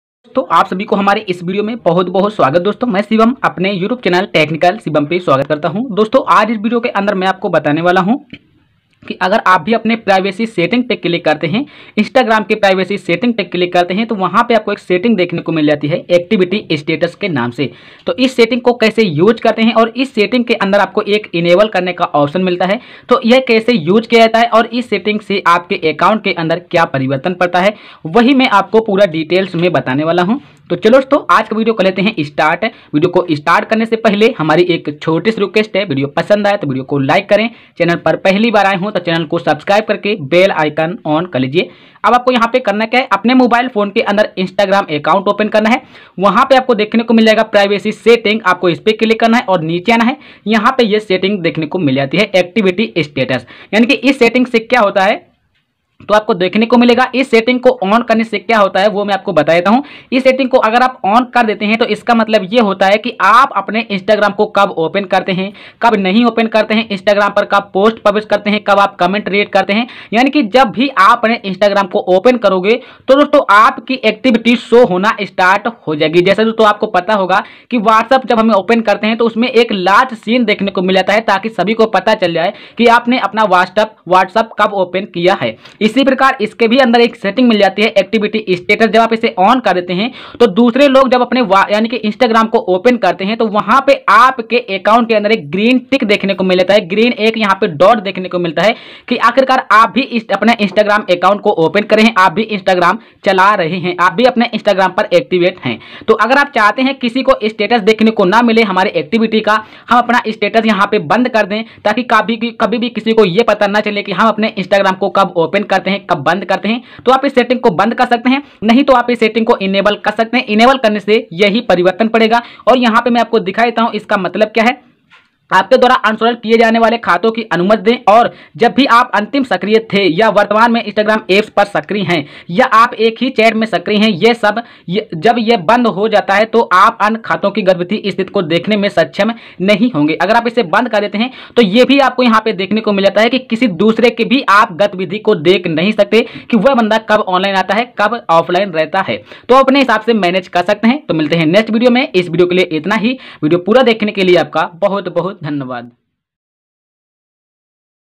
how to use activity status setting on Instagram privacy
how to use activity status setting on Instagram privacy || @Technical Shivam Pal

दोस्तों is video ke andar aapko bataya gaya hai ki Instagram ke a privacy setting main activity status setting kaise use karte hain is setting se Instagram ke privacy mein kya Parivartan padta hai video pura dekh Lijiye aap ko pata chal jaega.